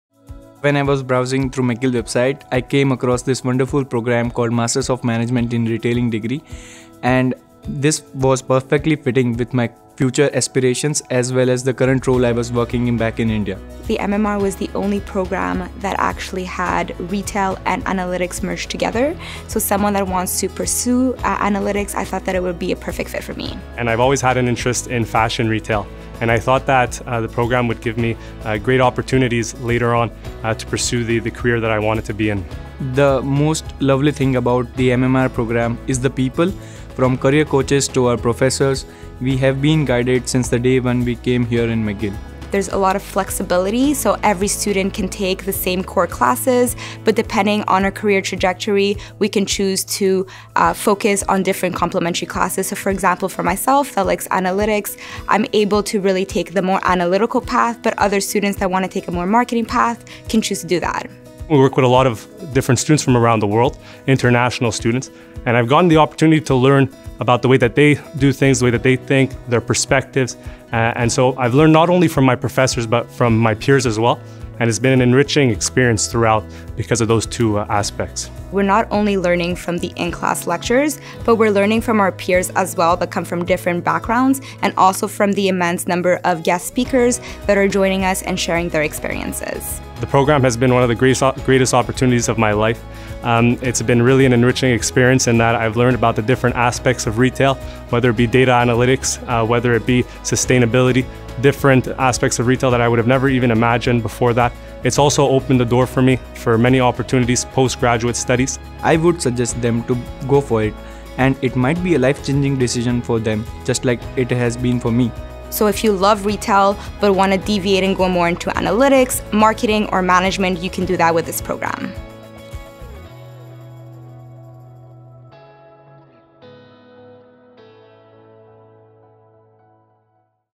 Why Choose the McGill Desautels MMR Program
Hear from Arman Mansuri, Vicky Constantopoulos, and Mikael Recine about why they choose to pursue the Master of Management in Retail (MMR) at the Bensadoun School of Retail Management (BSRM) at McGill Desautels.